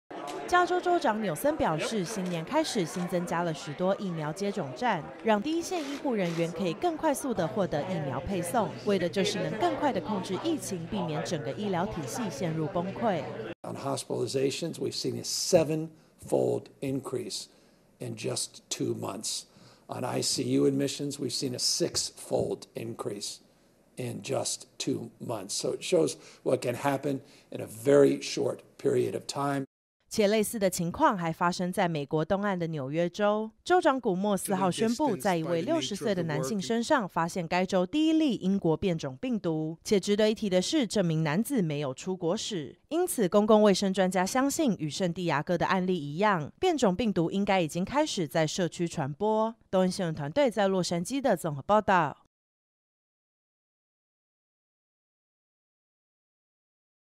加州住院與病床位 兩個月內需求爆增
NY首例英國變種病毒 LA醫療系統癱瘓邊緣

美國 加州與紐約，4號週一上午分別召開「州長記者會」，說明當前疫情的 嚴重程度，不只紐約州發現 首例英國變種病毒，加州人口最多的 洛杉磯縣，醫療體系也 接近癱瘓。

加州州長 紐森表示，新年開始，新增加了許多「疫苗接種站」，讓第一線醫護人員，可以更快的獲得 疫苗配送，為了就是能更快的 控制疫情，避免整個醫療體系、陷入崩潰。

且類似的情況，還發生在 美國東岸的 紐約州，州長古莫 4號宣布，在一位六十歲的男性身上，發現該州第一例「英國變種病毒」。

且值得一提的是，這名男子 沒有出國史，因此，公共衛生專家相信，與聖地牙哥的案例一樣，變種病毒應該已經開始在「社區傳播」。

東森新聞團隊 洛杉磯報導